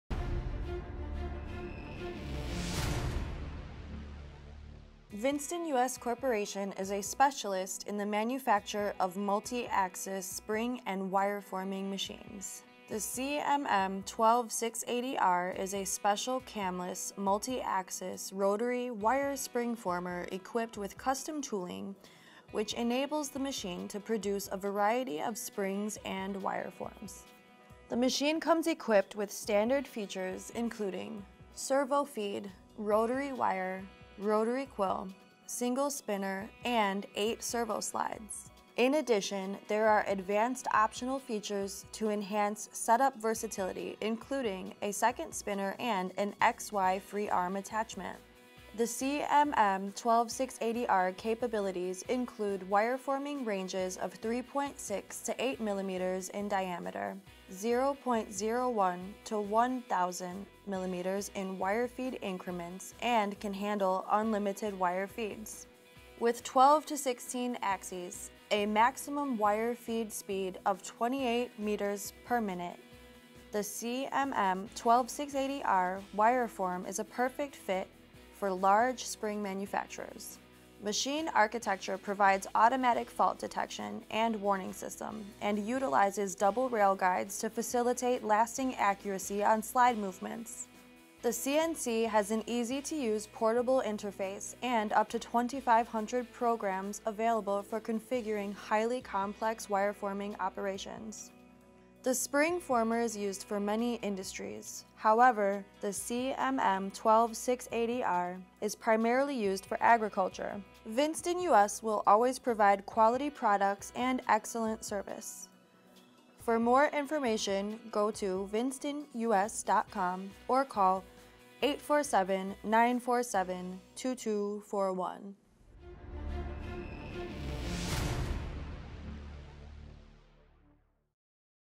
CMM-12-680 R Wire Spring Former CNC Capabilities Video VINSTON US CORP 2019
Special design equipped with:

• Servo Feed
• Rotary Wire
• Rotary Quill
• Single Spinner
• Optional Second Spinner
• 8 Servo Slides
• Optional X-Y Slide  (X-Y free arm that moves perpendicular to servo slide – 5.0mm Max Wire Limit)

1. A special Camless design equipped with custom tooling enables our series of spring machines to easily produce a variety of springs and wire forms.
2. Utilizes double rail guides to facilitate lasting accuracy on slide movements.
3. Advanced attachments such as an x-y free arm and a second spinner tool can be optionally equipped.
4. Machine architecture provides automatic fault detection and warning system.
5. Dual spinner support and an x-y free arm attachment add versatility to setups.

Specifications and parameters (1 inch = 25.4 mm):
MODEL CMM-12-680 R Wire Former
Wire Diameter 3.6-8.0mm (Spring Wire)
Maximum Wire Feed 1000.00mm
Minimum Wire Feed 0.01mm
Maximum Wire Feed Speed 28 m/min
Maximum Wire Feed Length Unlimited
Number Of Axis 12 (Up to 16)
Power 52.5 kW
Voltage 220V
Voltage Regulator Power 30 KW
Software Industrial Grade
Program Storage 2500
Air Compressor System Air pressure 5-6 kg/cm
Pin 3
Dimensions Depth 3600mm
Width 2220mm
Height 2310mm